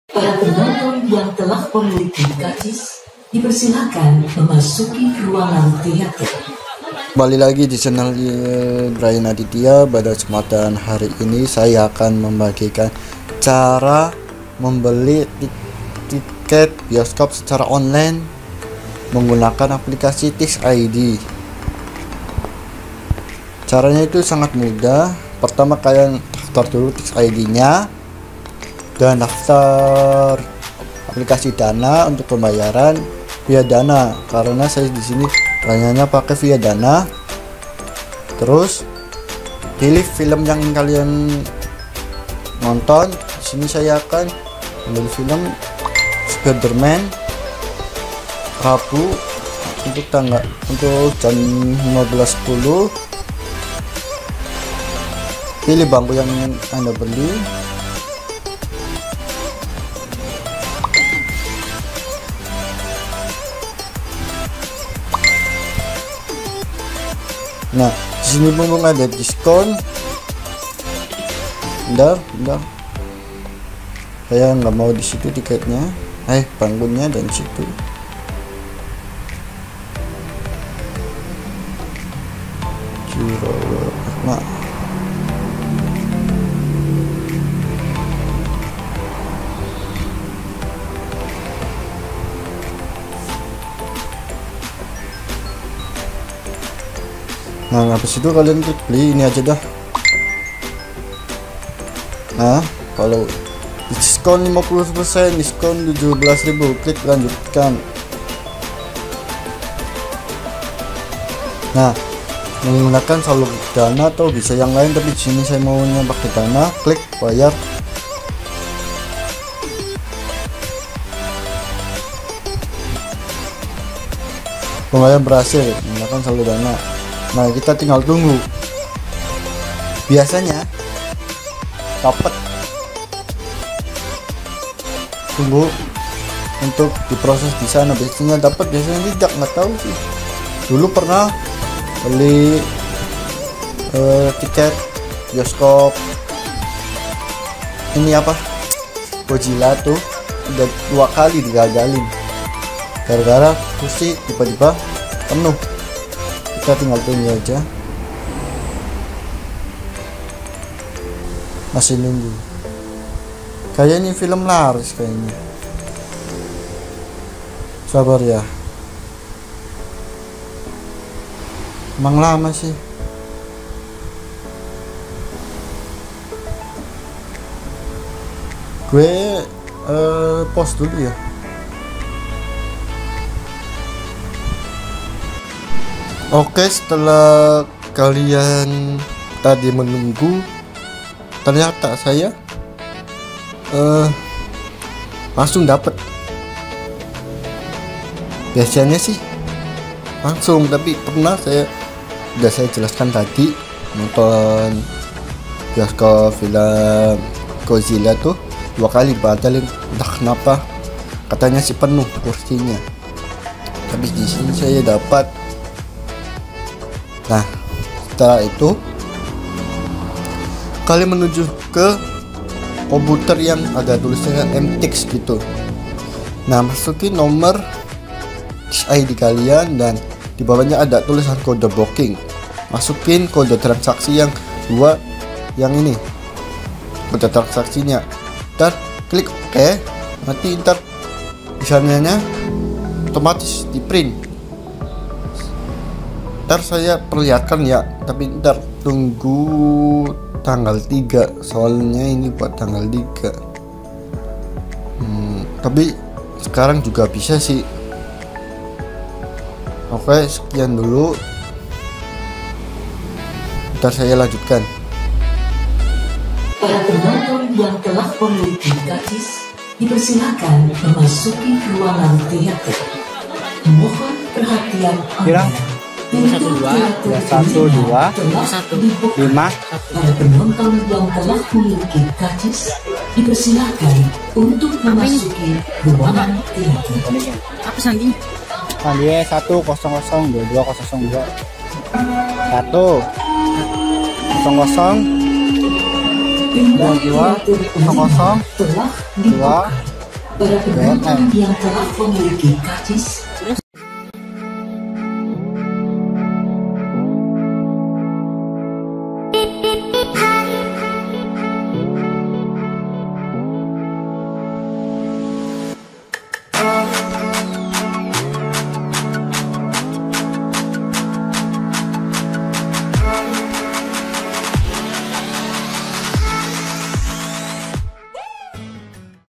Cara Membeli Tiket Bioskop Di TIX ID Bayar Lewat Dana
Bagi kalian Ingin membeli Tiket Bioskop Secara Online dan ingin mendapatkan banyak keuntungan dari pada beli di offline bisa menggunakan video ini, beli tiket bioskop secara online tidak perlu antri lagiih broo.